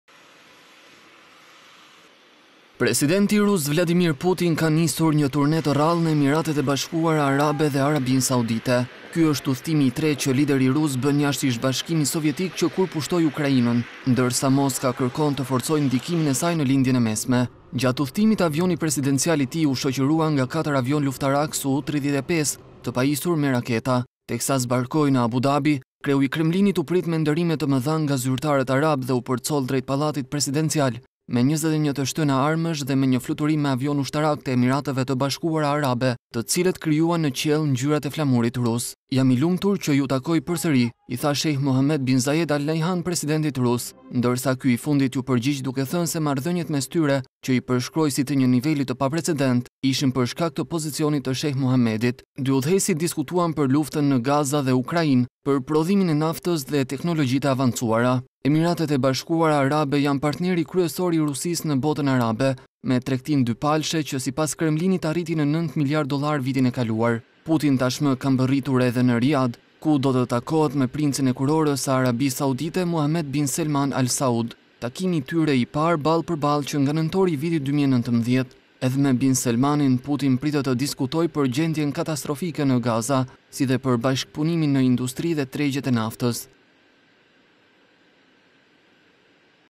Putin nis turin në Lindjen e Mesme, pritet me nderime të mëdha në Emiratet Arabe | RTSH
Gazetare: Artur Mata
Pamje: AFP